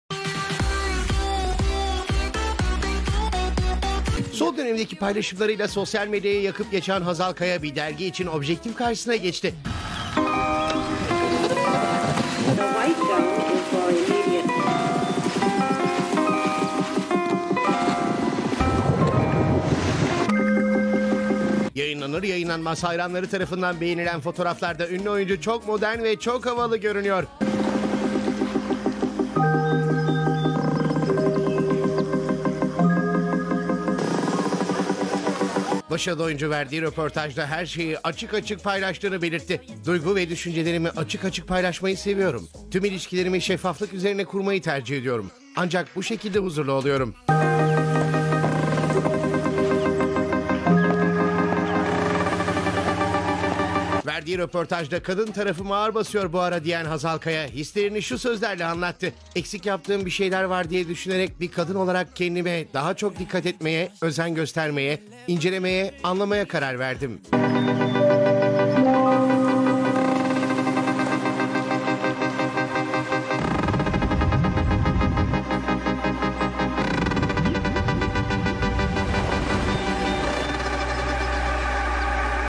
Ünlü oyuncu Hazal Kaya, özel hayatında nasıl bir insan olduğunu anlattı.
2019 yılında evlenen ve Ali Atay ile mutlu bir evliliği olan Hazal Kaya, bir derginin Temmuz sayısı için röportaj verdi.

Oyuncu Hazal Kaya hem özel hem de iş hayatında cesur ve şeffaf biri olduğunu söyledi.

"Açık açık paylaşırım"
Elle dergisine röportaj veren Hazal Kaya, "Duygu ve düşüncelerimi açık açık paylaşmayı seviyorum. Tüm ilişkilerimi şeffaflık üzerine kurmayı tercih ediyorum. Ancak bu şekilde huzurlu oluyorum" dedi.